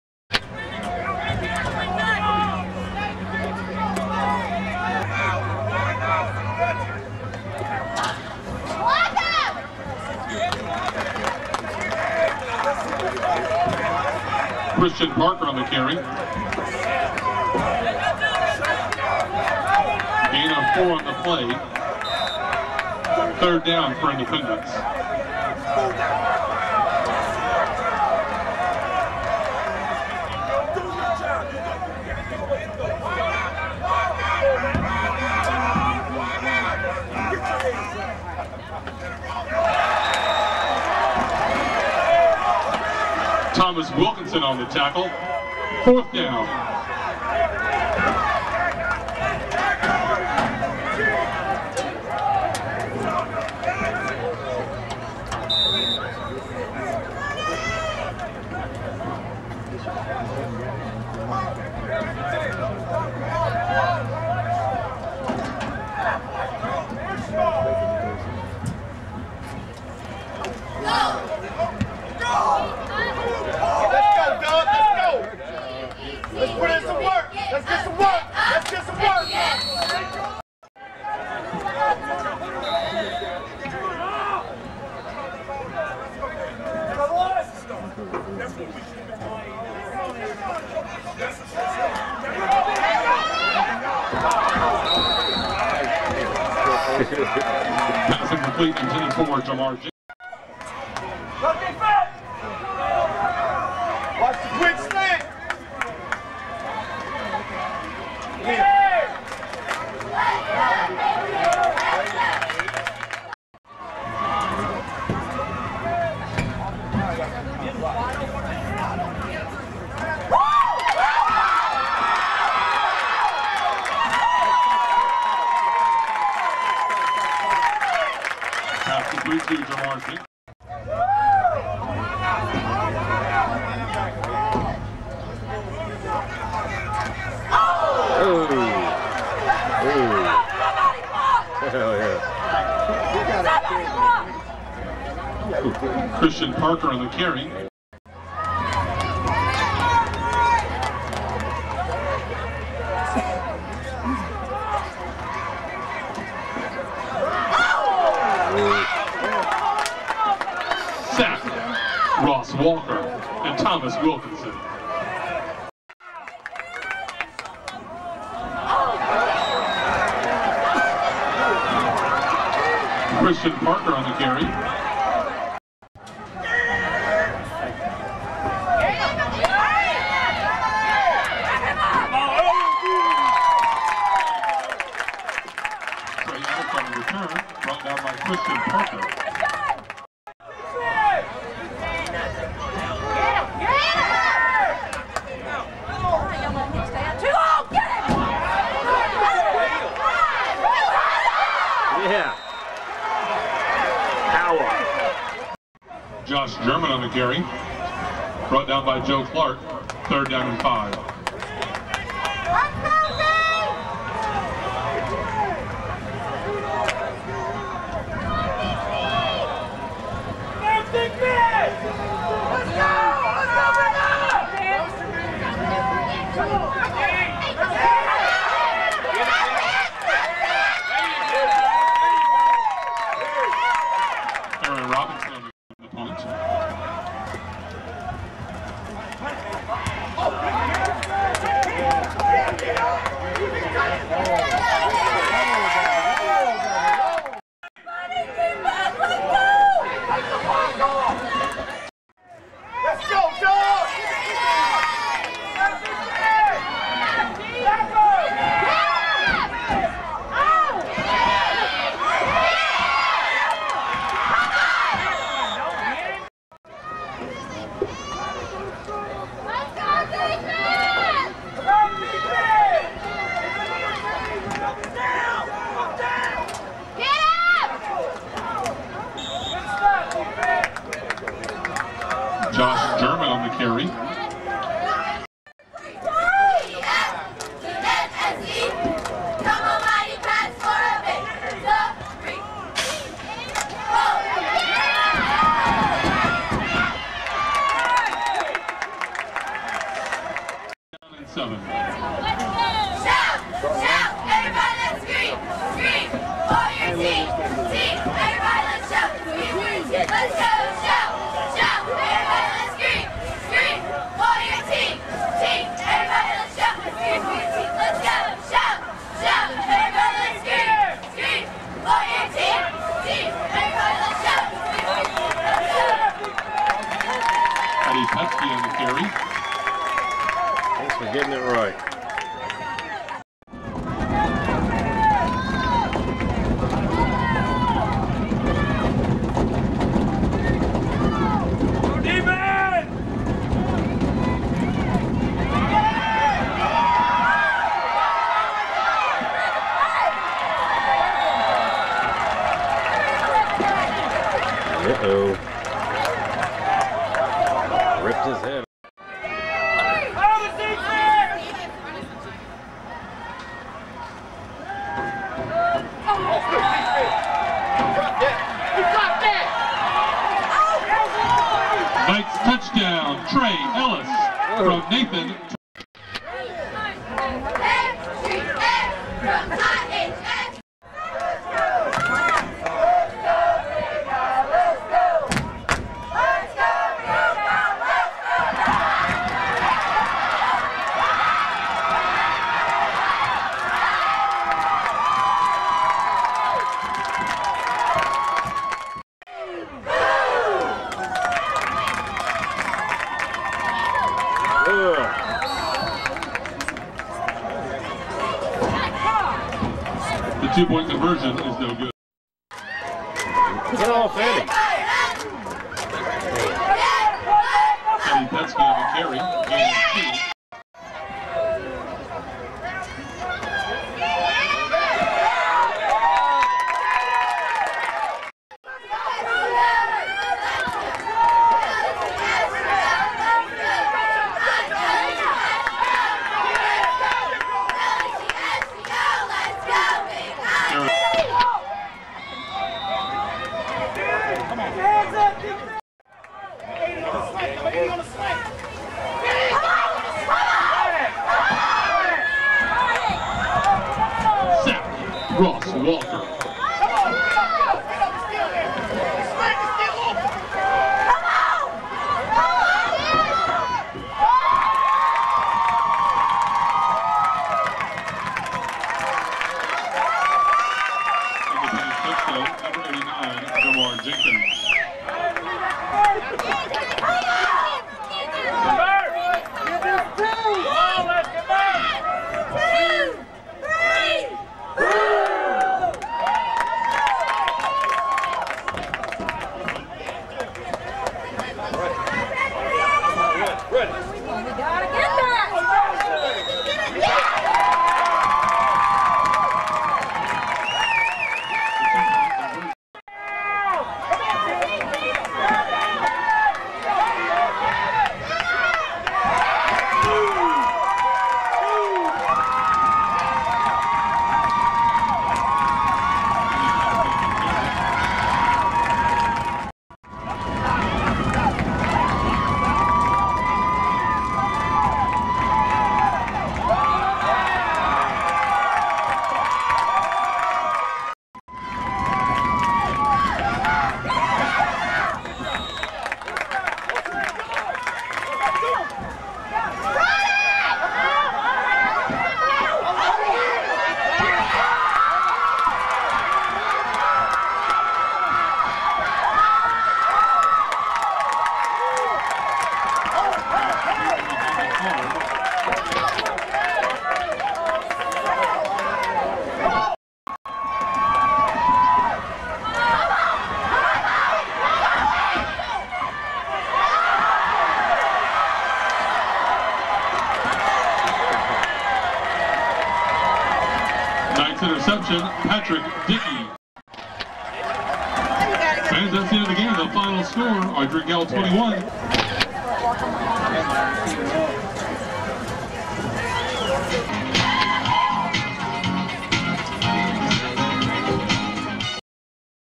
Ardrey Kell vs Independence JV High School Football 9/22/11
Game Highlights.  Congratulations to Ardrey Kell for beating Indy for the first time ever.